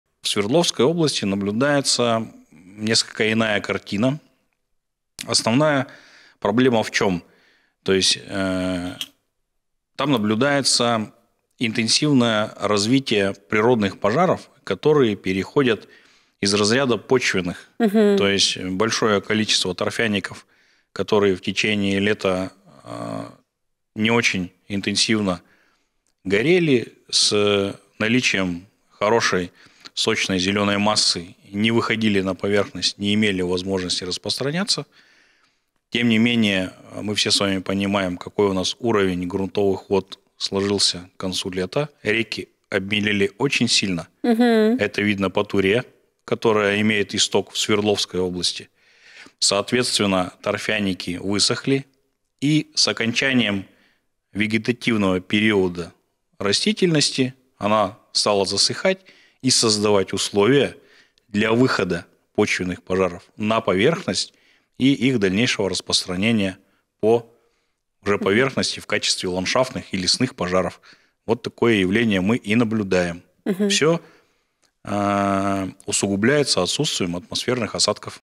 Запах гари в Тюмени
Запах гари пришёл в Тюмень из Свердловской области. В чём причина лесных пожаров и почему их так сложно потушить?